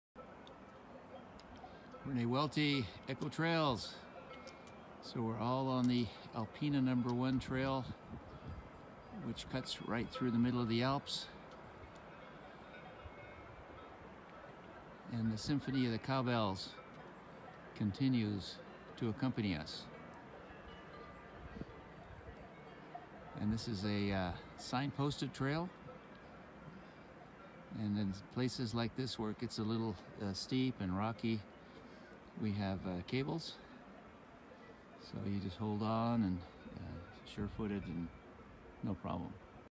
Via Alpina Swiss Hiking Trail
This the segment between the alpine village resorts of Engelberg & Melchsee-Frutt and part of the Echo Trails Swiss Alps 8-Day tour.